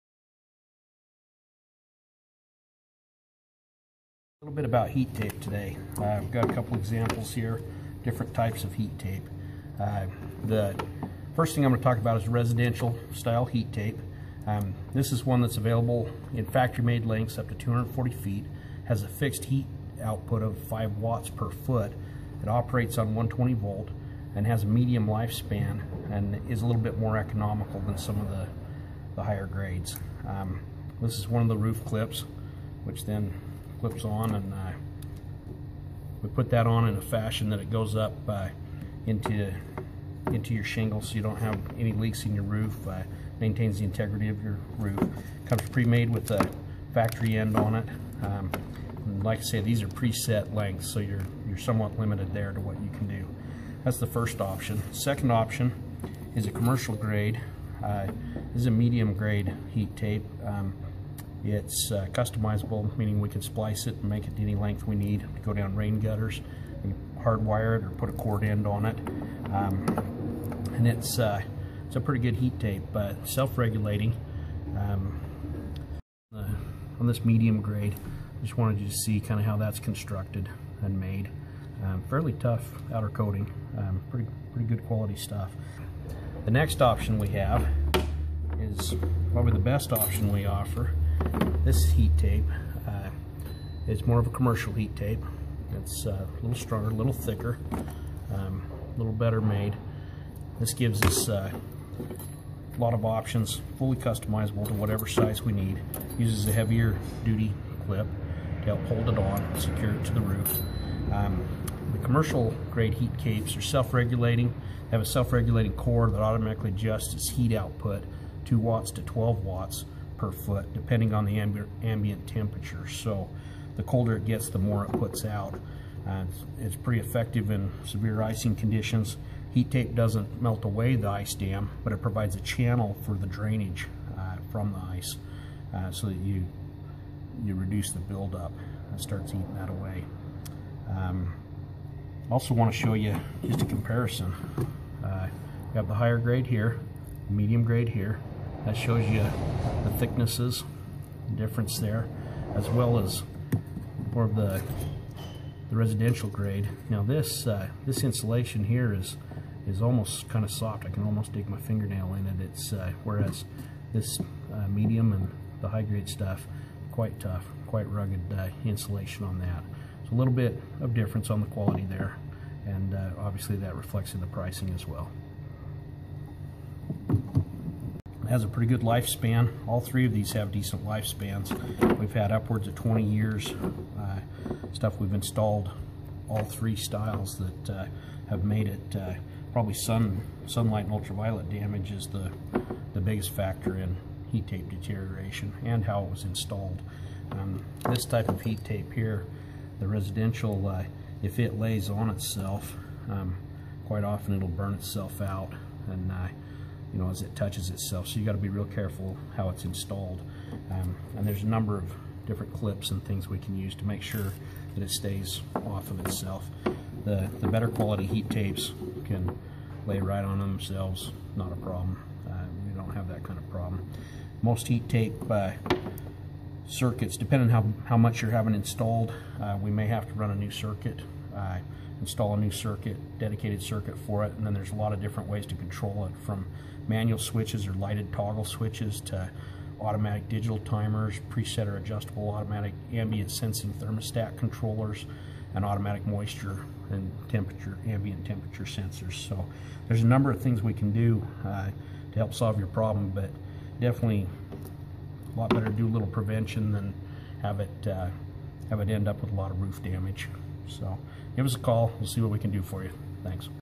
Roof Heat Cable, Heat trace, or Ice melt cable types
Examples of types of roof heat cable to prevent ice dams on roofs and rain gutters.